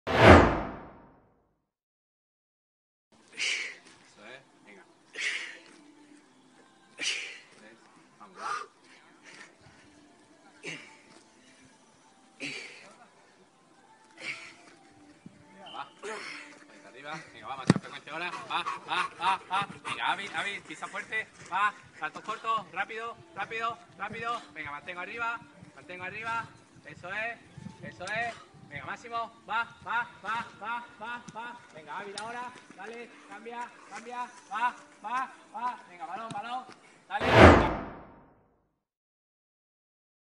El chileno Pávez sigue entrenándose al margen del grupo en el Cádiz
El chileno Pávez sigue entrenándose al margen del grupo en el Cádiz. Imágenes de ElDesmarque.